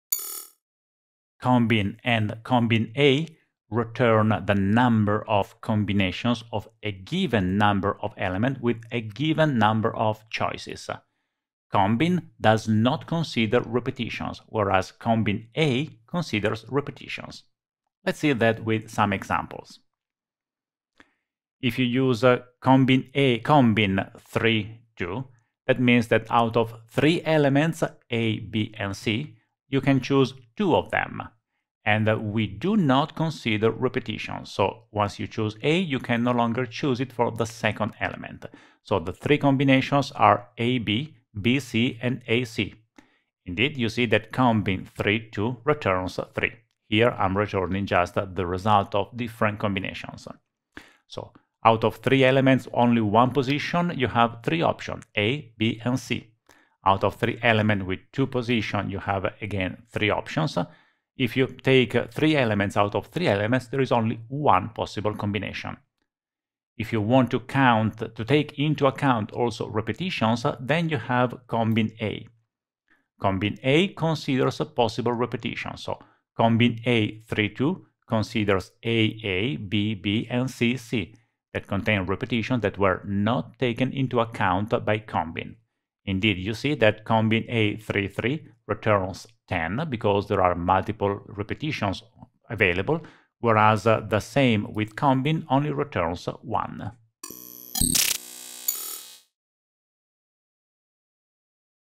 COMBIN, COMBINA - DAX Guide
COMBIN: Returns the number of combinations for a given number of items.

COMBINA: Returns the number of combinations (with repetitions) for a given number of items.